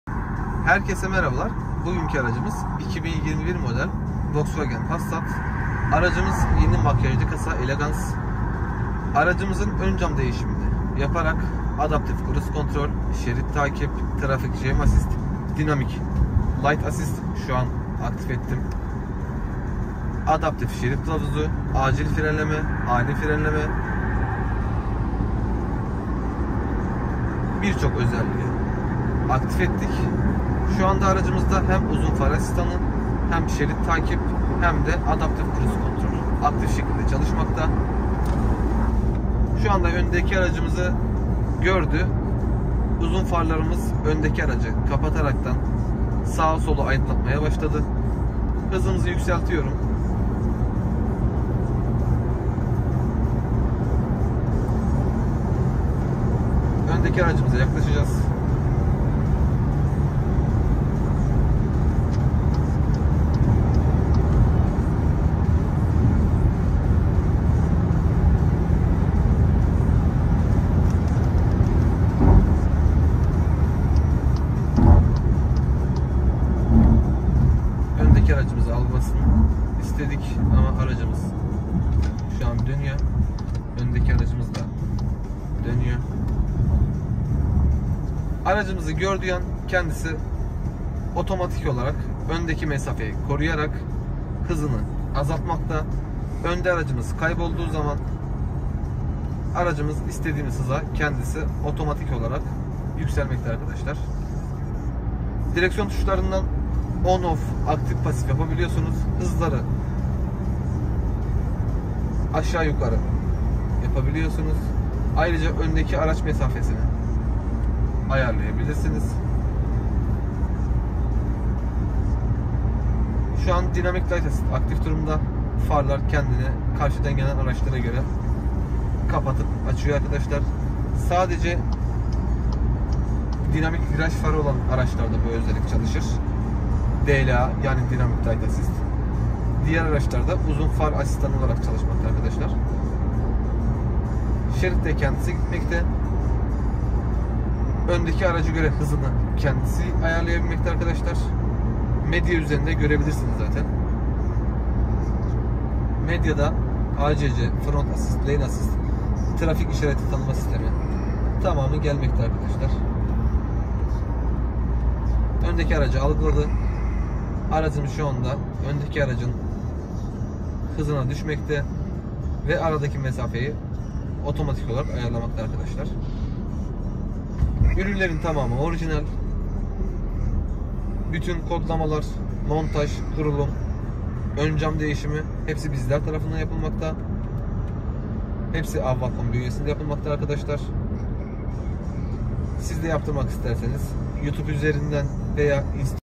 2021 PASSAT ELEGANCE PART 2 - ACC-LANE ASS-DLA-EMERGECY ASS-TJA-HİGH BEAM ASS-FRONT ASS-
🧨🧨2021 MODEL PASSAT ELEGANCE Predictive Adaptive Cruise Control💣💣

👉Montajı Yapılan Donanımlar ACC+ LANE ASSİST

1️⃣ ACC
✅Adaptif Cruise Control
✅Ön Bölge Çarpışma Önleyici

2️⃣LANE ASSİST
✅Şerit Takip Asistanı
✅Uzun Far Asistanı
✅Tabela Okuma
✅Ani Frenleme

👉ACC VE LANE ASSİST BERABER MONTE EDİLDİĞİNDE EKSTRA İKİ ÖZELLİK GELMEKTEDİR...

✅Trafik Sıkışıklığı Asistanı
✅Acil Durum Asistanı
✅Dinamik light assist

 Hayırlı Olsun😊